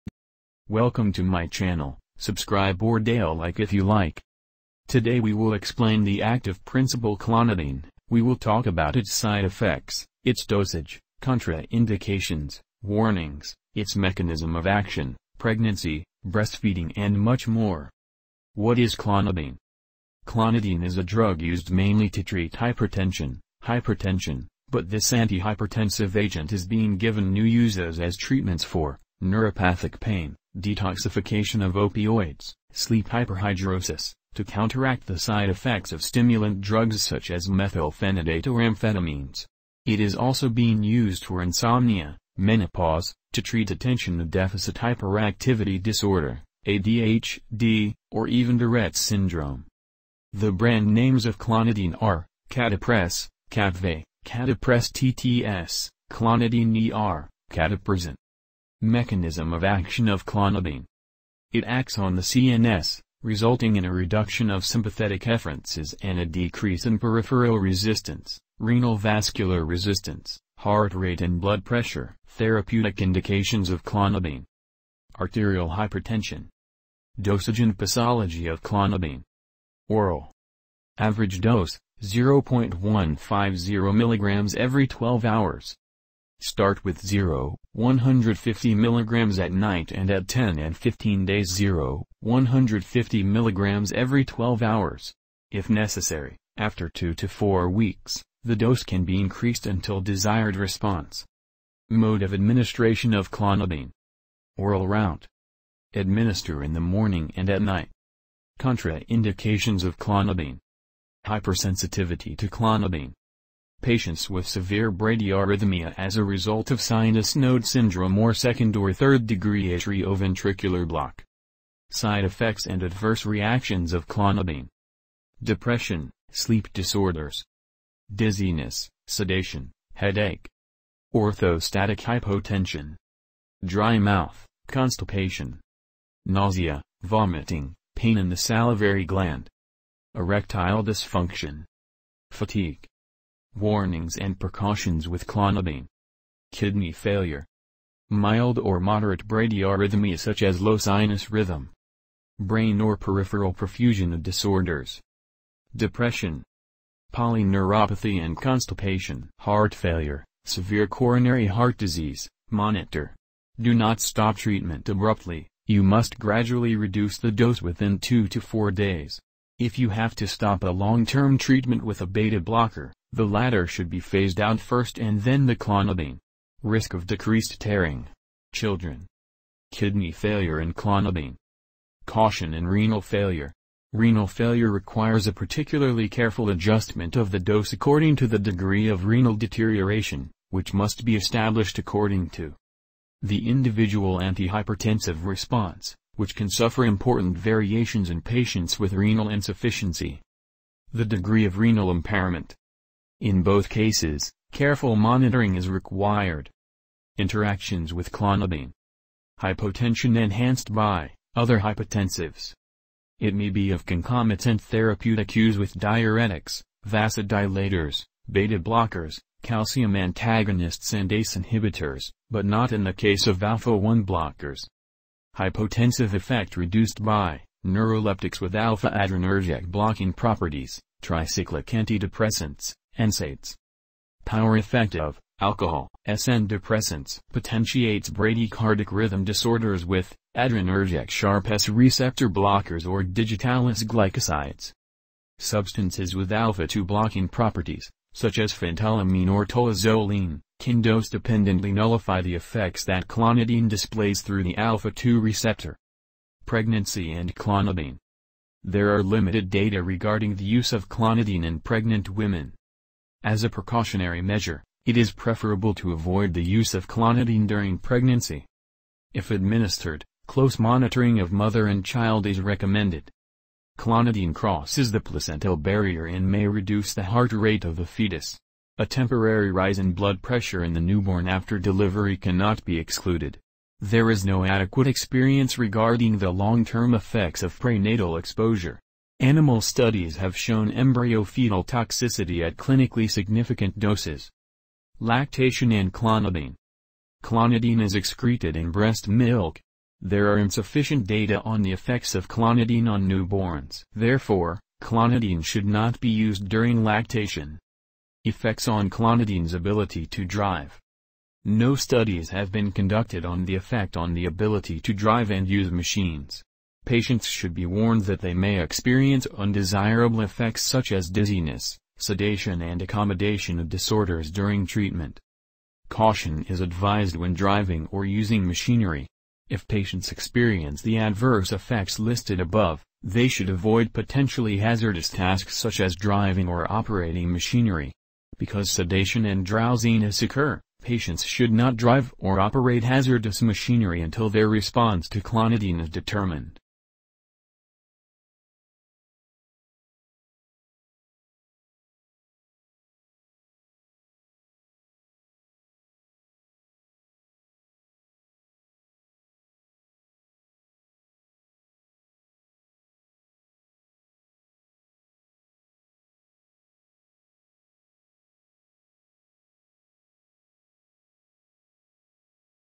CLONIDINE: Uses | Dosage | Side Effects | Hcl | clonidine withdrawal
💊All about clonidine medicine: clonidine dosage (0.1 mg, 2 mg), clonidine side effects, clonidine uses, clonidine hcl, clonidine withdrawal.
Clonidine is a drug used primarily to treat hypertension blood pressure (HBP), but this antihypertensive agent is being given new uses as treatments for: neuropathic pain, detoxification of opioids, sleep hyperhidrosis, to counteract the side effects of stimulant drugs such as methylphenidate or amphetamines. It is also being used for insomnia, menopause, to treat attention deficit hyperactivity disorder (ADHD) or even Tourette's syndrome.

The brand names of Clonidine are: Catapres, Kapvay, Catapres-TTS, Clonidine ER, Catapresan.